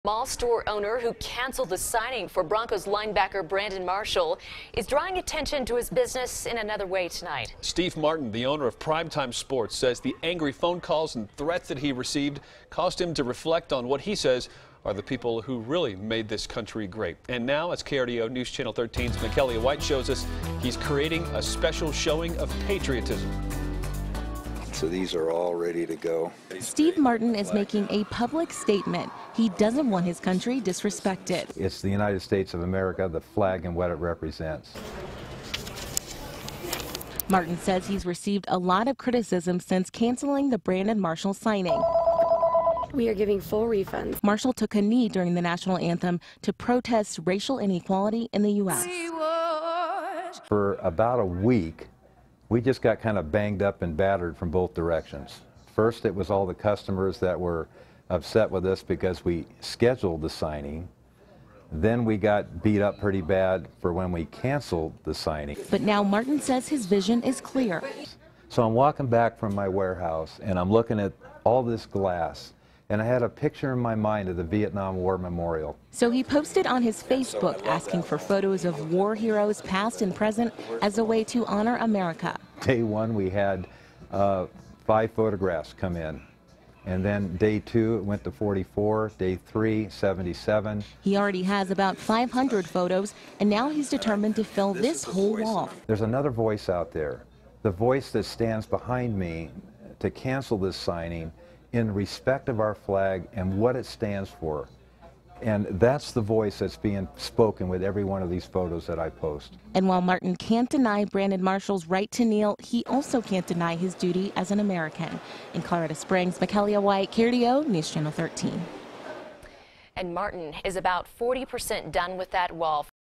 Chapel Hills Mall store owner creates wall of photos to show
Chapel Hills Mall store owner creates wall of photos to show patriotism